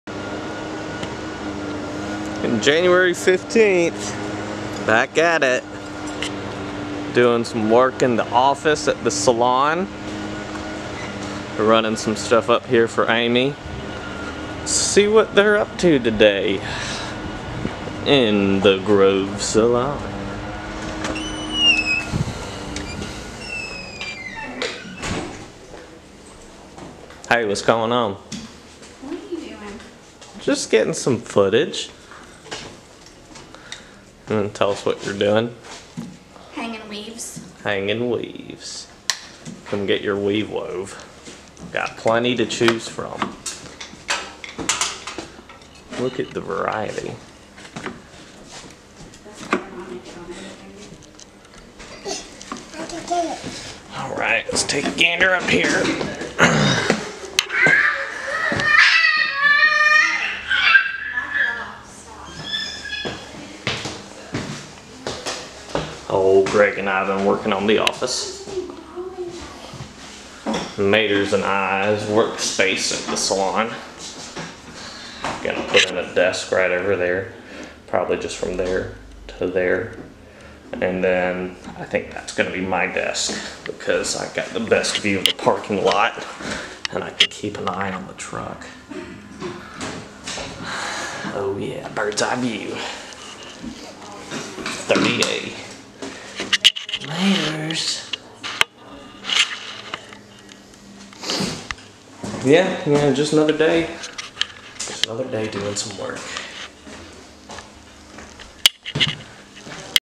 Dads that vlog (episode 007) The Grove Salon in Grayton beach on 30A
Running around down to my wifes Salon in Grayton beach. Check out the Grove Salon in Grayton beach on 30A Santa Rosa Beach Florida